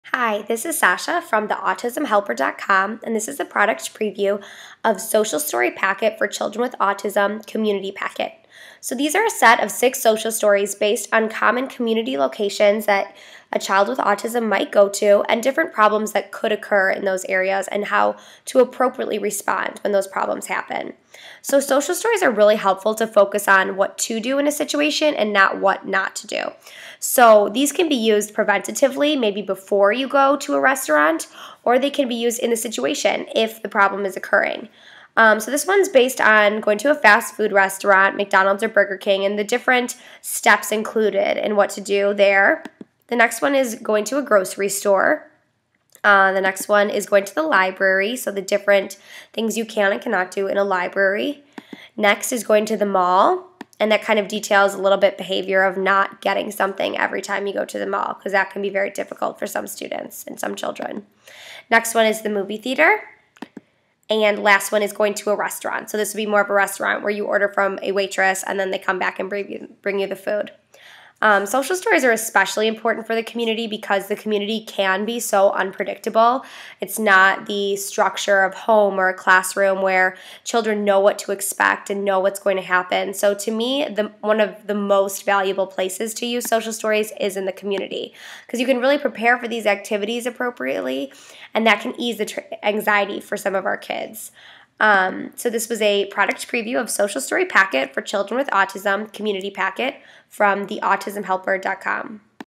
Product Preview - Visual Social Story Packet - Community Set
Product Preview for Visual Social Story Packet for Children with Autism - Community Set by theautismhelper.com.